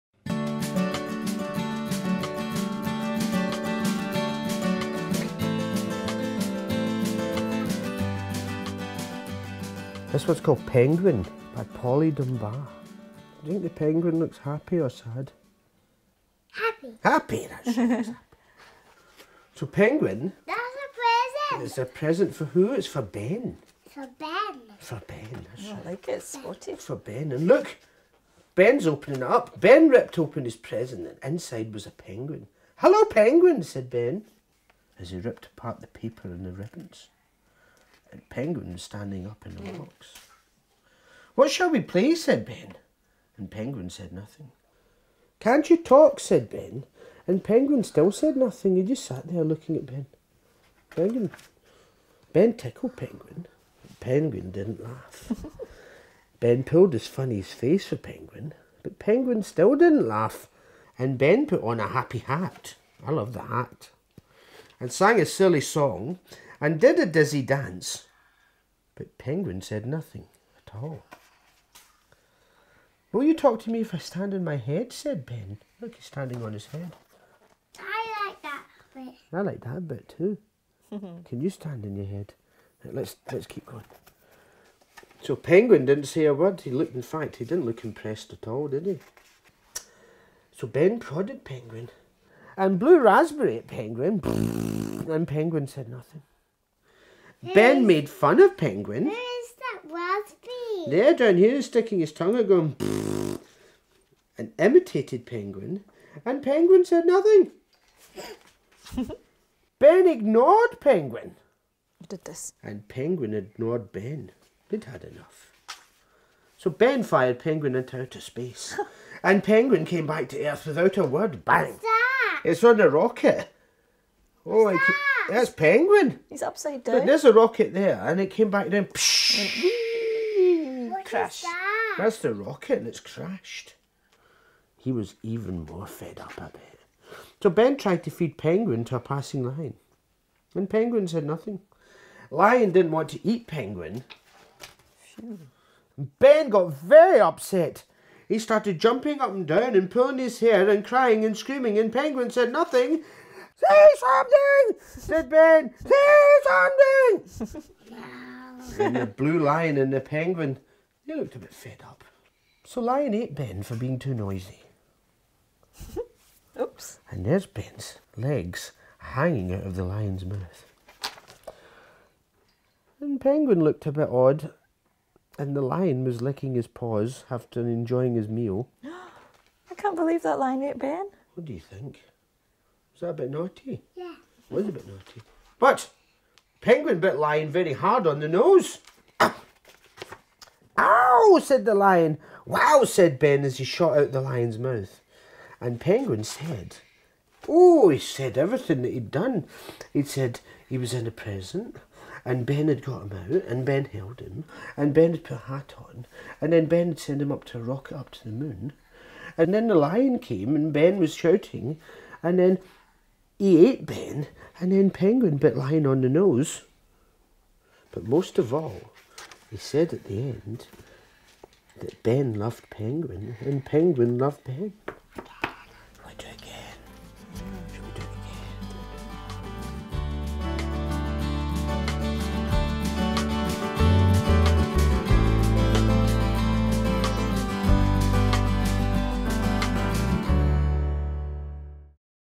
Penguin by Polly Dunbar | Bookbug Bag Books
Did you receive Penguin by Polly Dunbar in your Bookbug Bag? Enjoy watching this family reading the book for ideas on ways to share it.

Find more information about Bookbug and tips and advice on sharing books with small children,